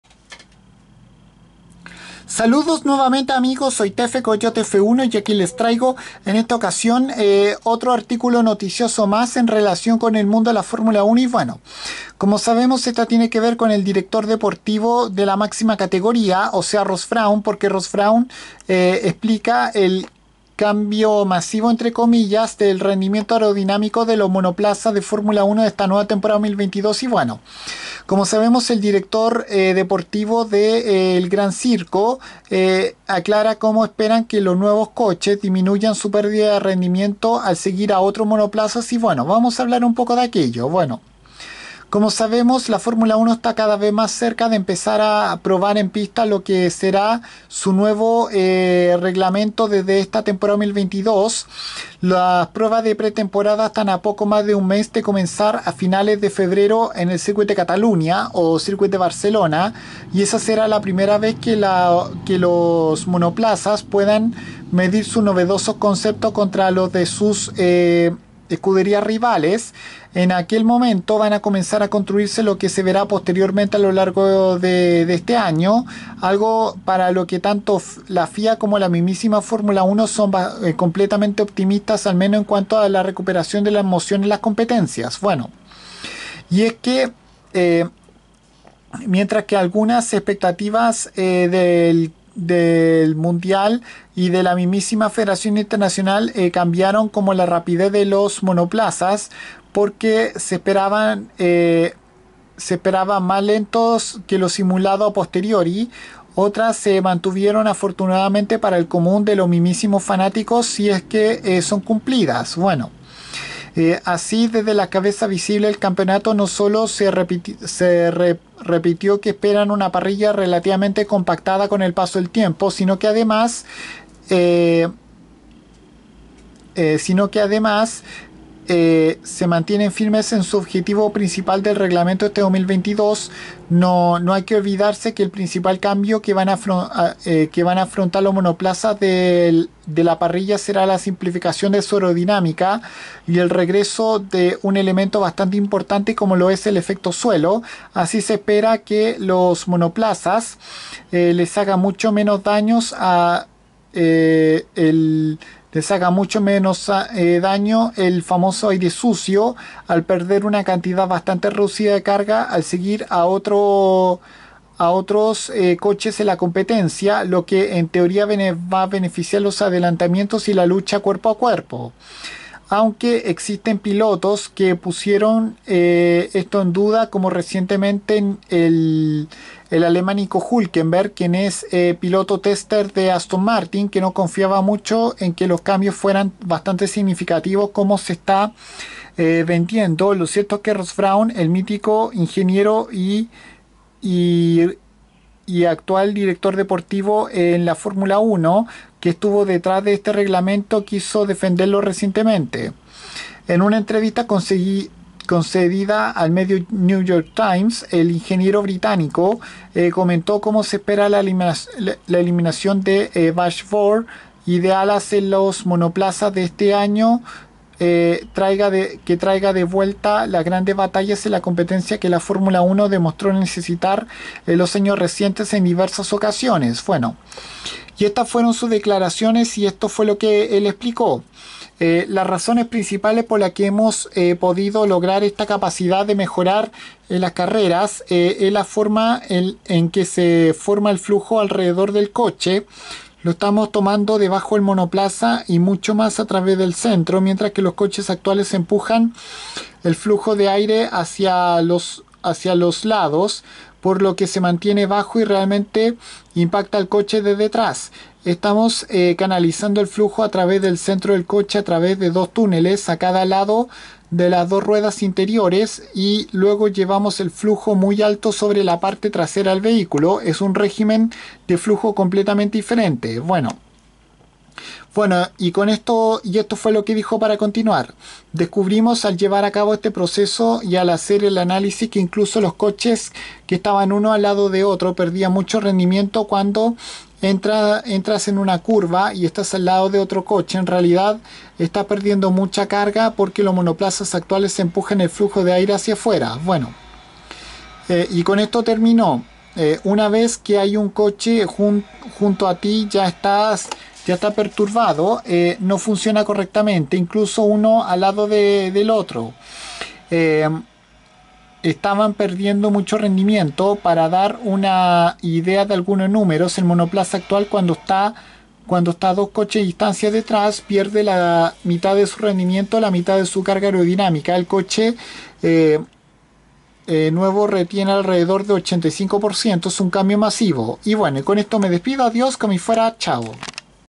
Ross Brawn explica como será el rendimiento aerodinámico de los nuevos F1 de 2022
El director deportivo de la categoría reina del automovilismo mundial aclara como se espera que los nuevos coches disminuyan su capacidad de perder rendimiento al perseguir a otros vehículos